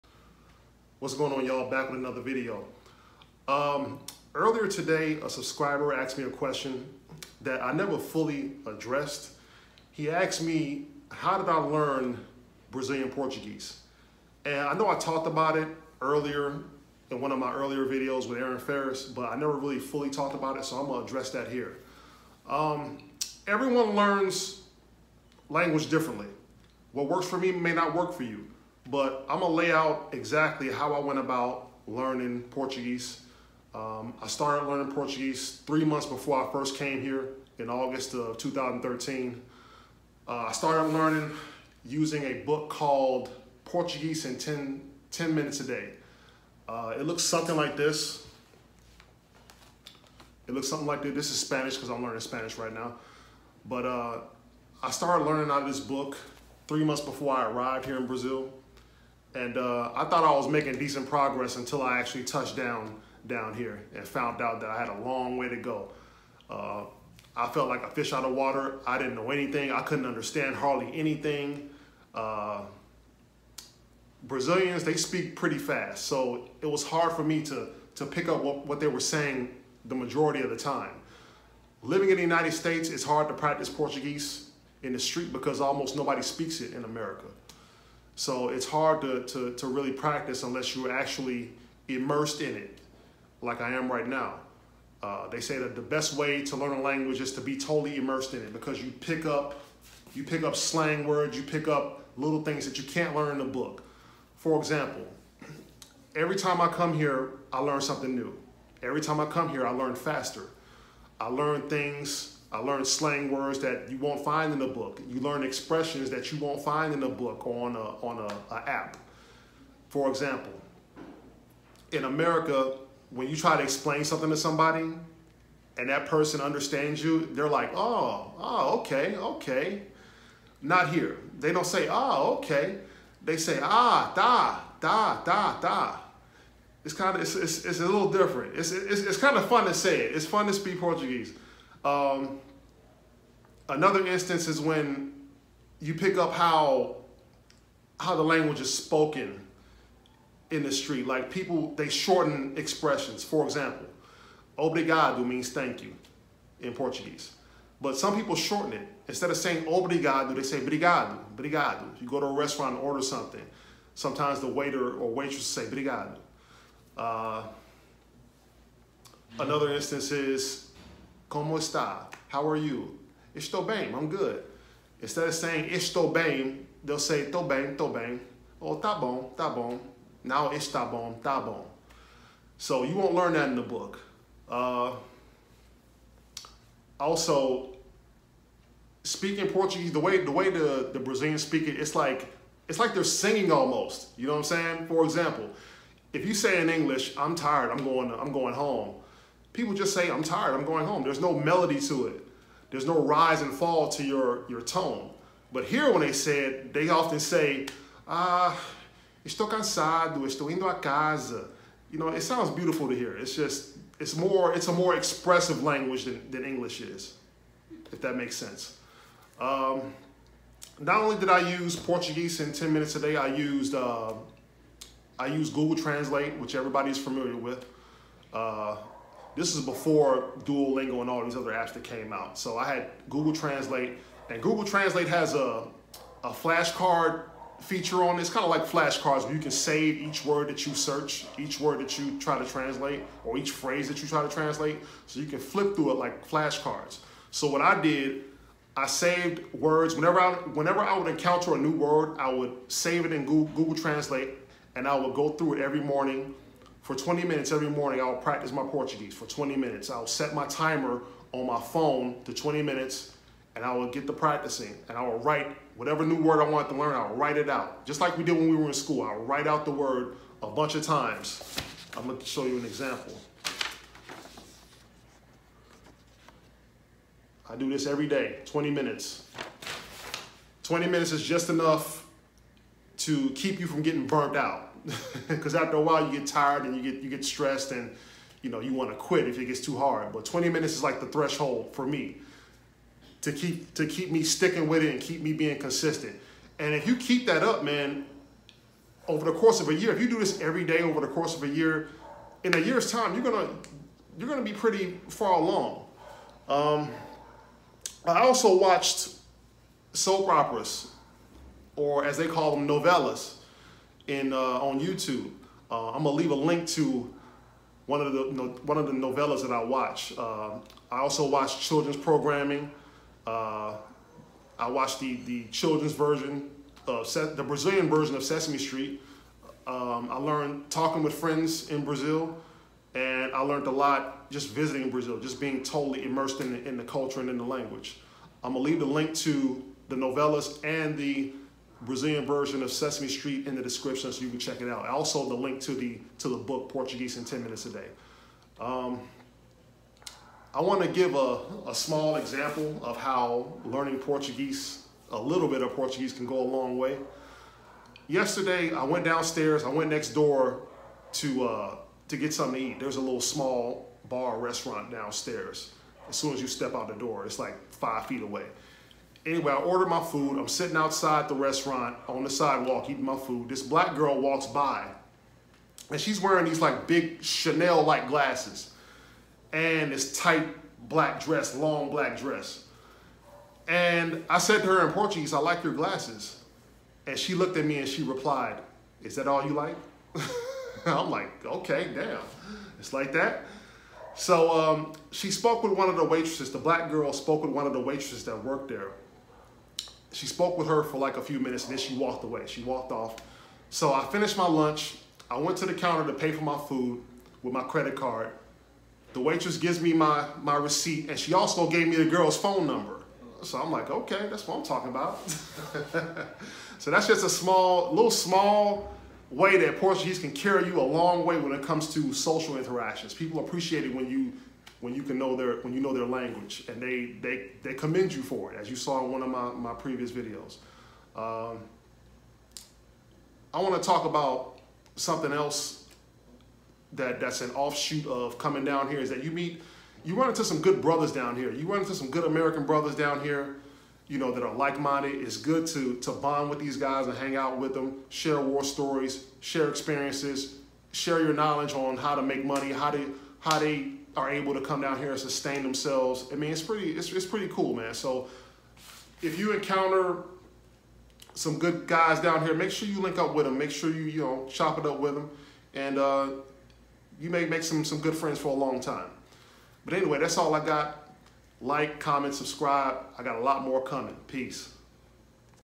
How I Learned Brazilian Portuguese
Here I talk about how I learned Brazilian Portuguese and how book learning is different from street learning.

PORTUGUESE in 10 minutes a day

Yahoo! Brazil

EQUIPMENT USED:
iPhone 8